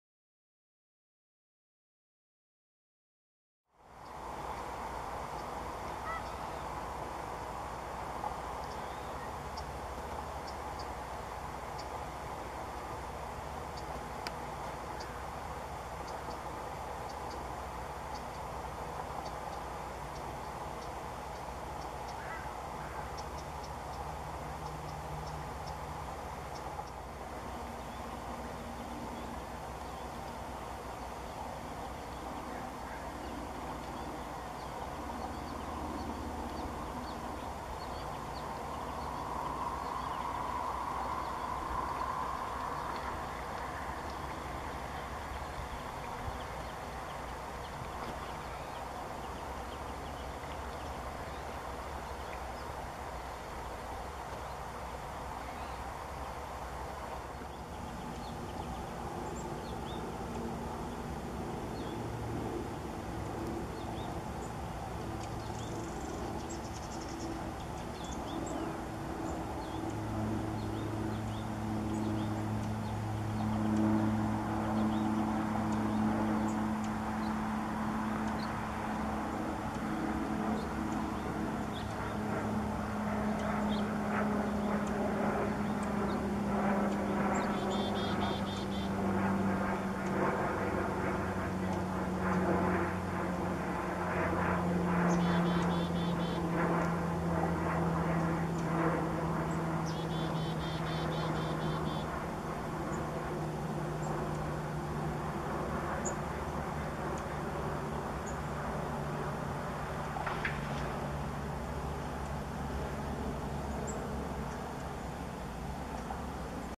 Yellowlegs
Greater and Lesser Yellowlegs showed up a week apart at Magnuson Park.